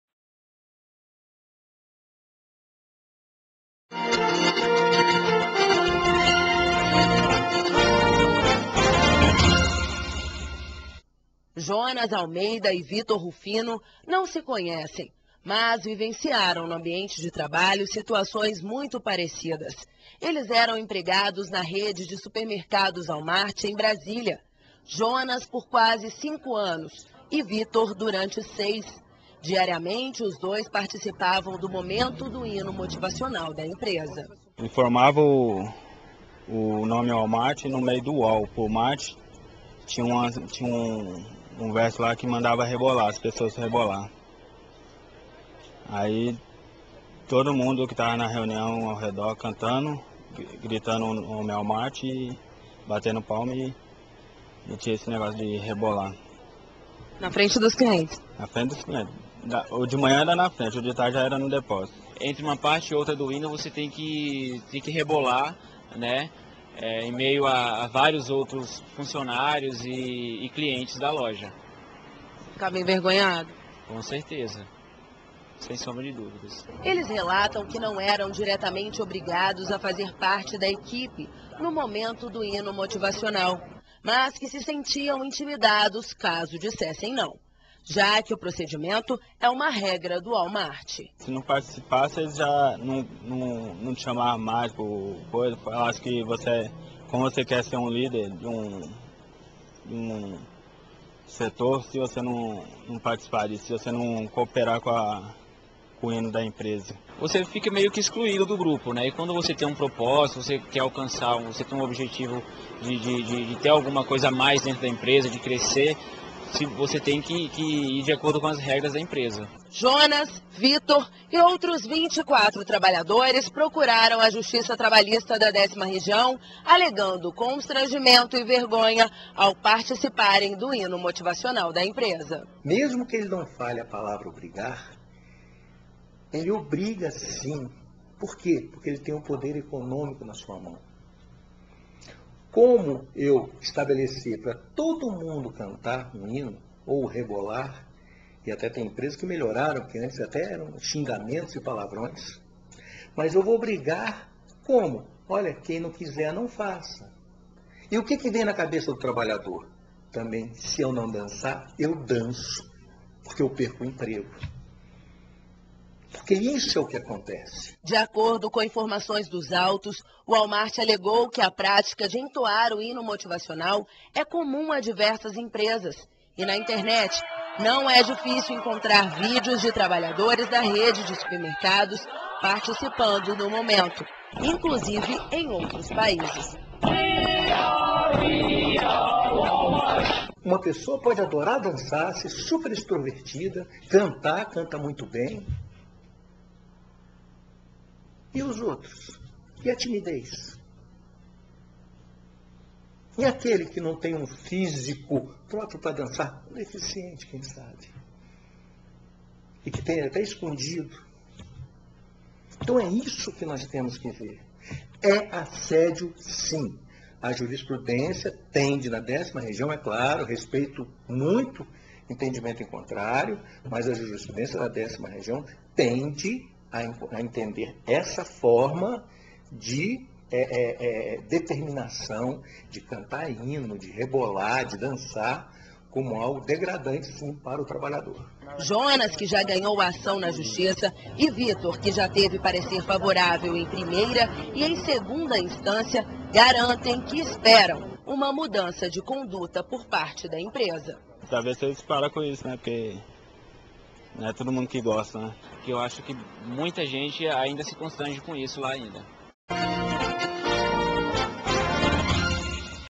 TV TRT10 - WalMart é condenado por obrigar funcionários a cantar hino motivacional
Walmart é acusado de obrigar funcionários a cantar hino motivacional e fazer dança constrangedora. Os casos chegaram à Justiça do Trabalho da Décima Região.